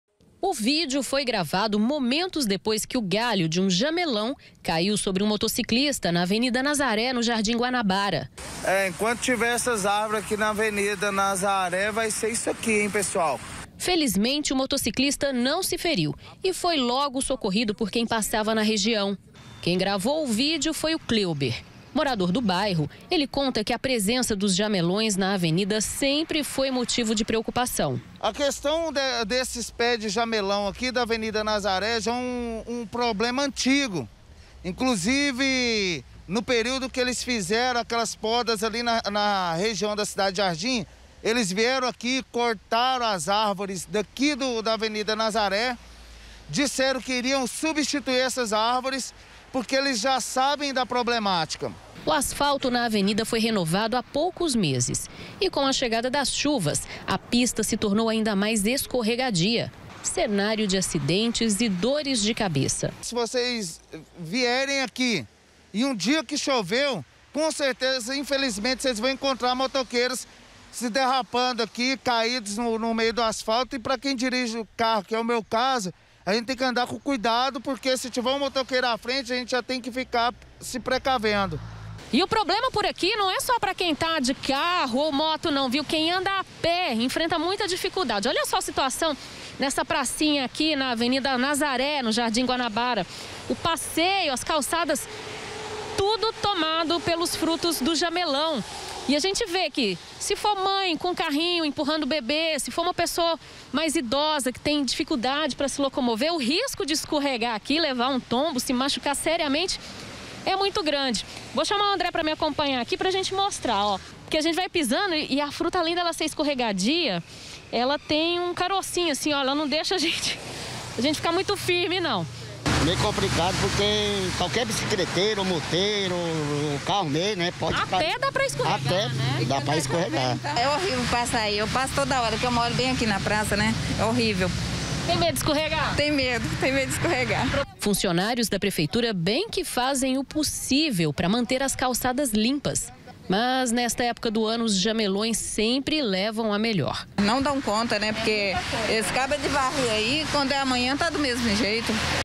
ÁRVORES SEM PODA: QUEDA DE GALHOS COLOCA MOTORISTAS EM RISCO
QUEM PASSA PELA AVENIDA NAZARÉ, NO JARDIM GUANABARA AQUI EM GOIÂNIA, ESTÁ PREOCUPADO COM A QUEDA DE GALHOS E FRUTOS, ONDE OS PÉS DE JAMELÃO ESTÃO SEM PODA. RECENTEMENTE, UM MOTOCICLISTA FICOU FERIDO DEPOIS DE SER ATINGIDO POR UM GALHO QUANDO PASSAVA PELO LOCAL.